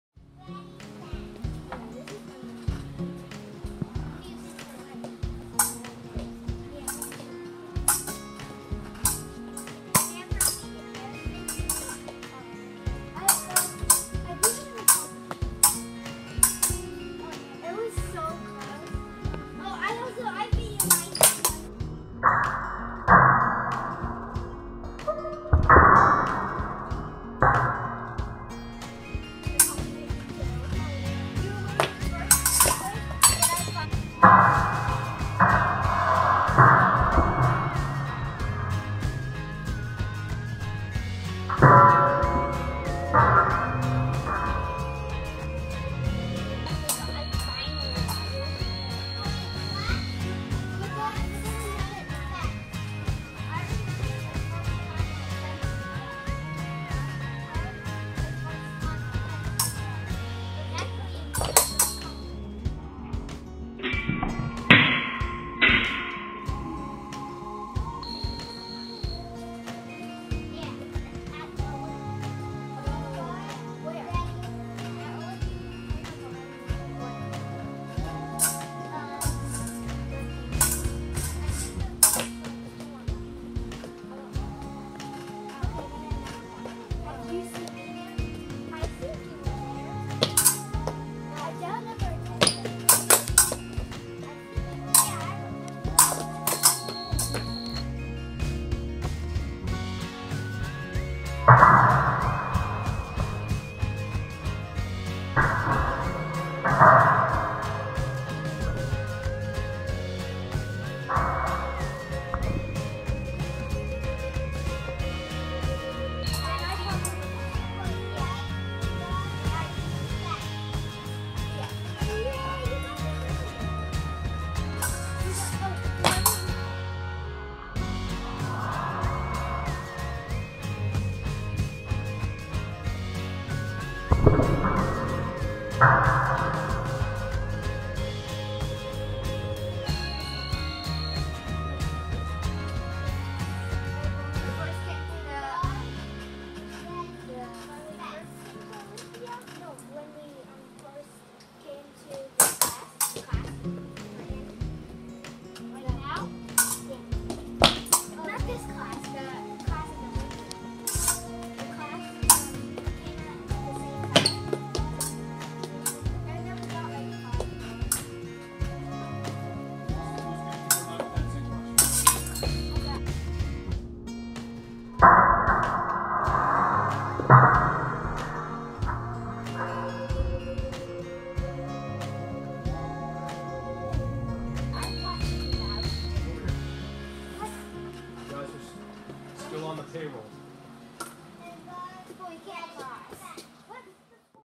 Harrison v. Daniel Epee fencing: Individual notes
This video points outs some areas of focus for the young fencers